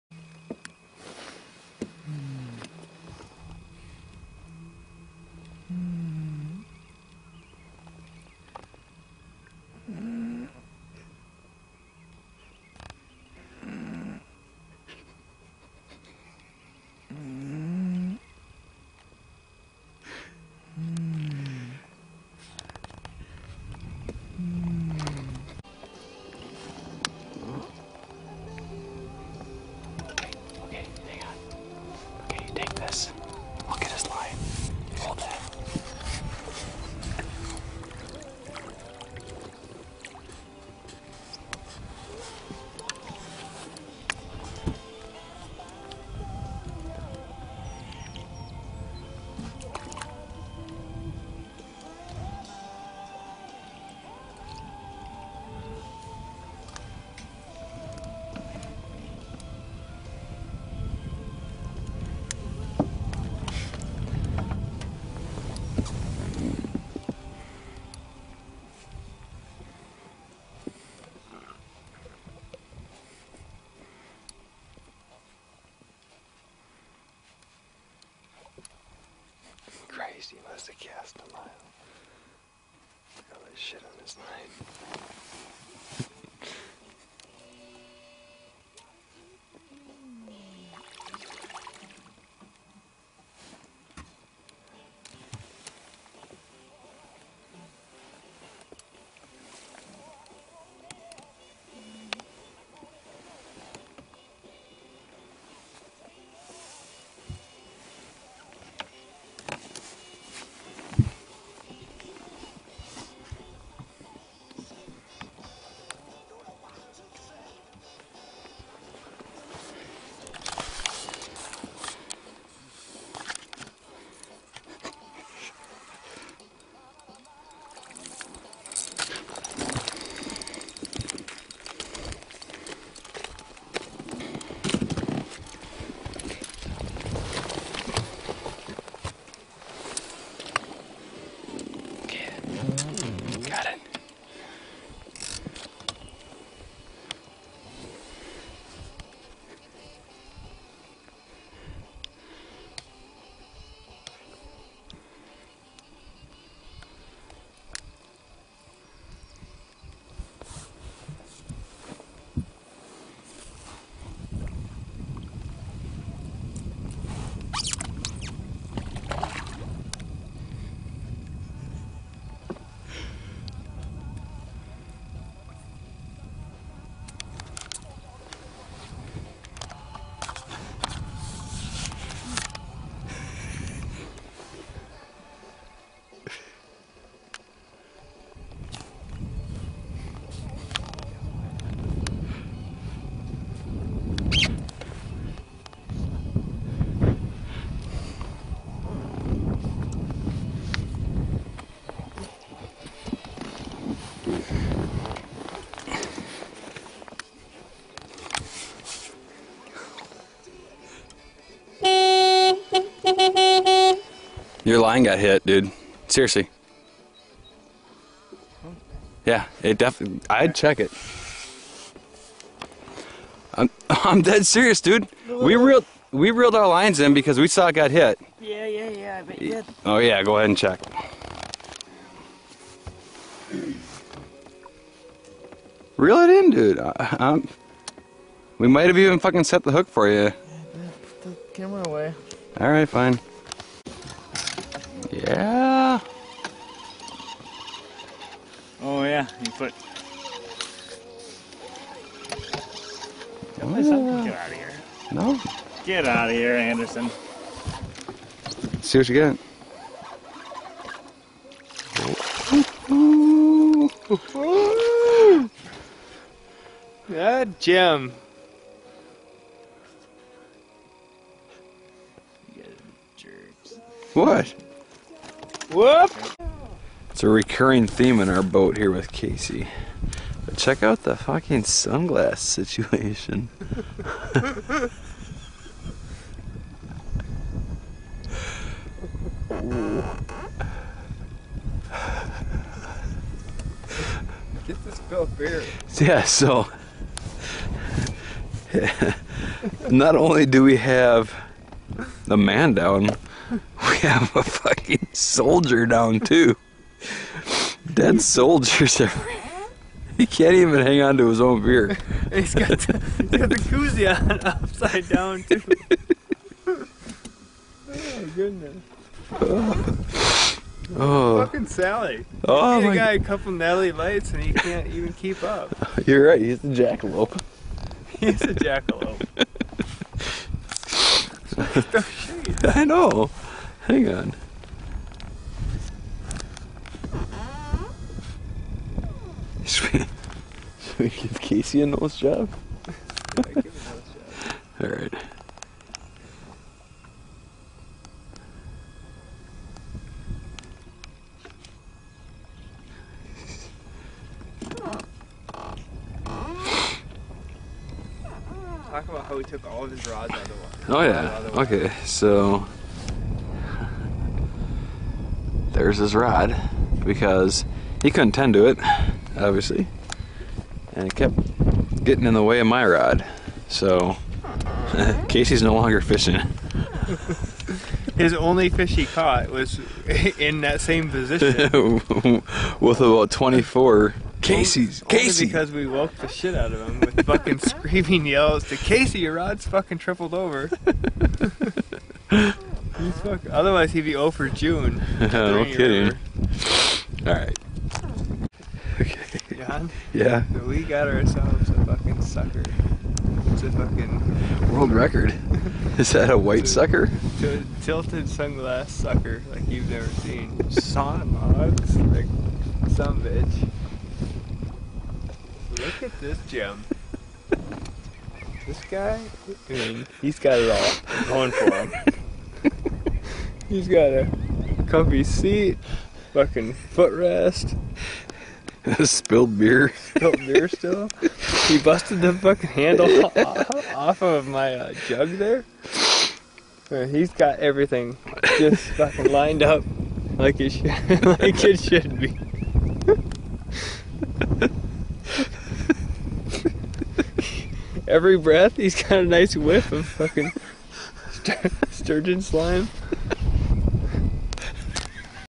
Casey - Do Not Fall Asleep Sturgeon Fishing
Casey falls asleep and Andy and I giggle and dink with him.  Clearly a poor sturgeon bite happening...
A new clip starts around the 4:35 timeframe that is pretty dang funny.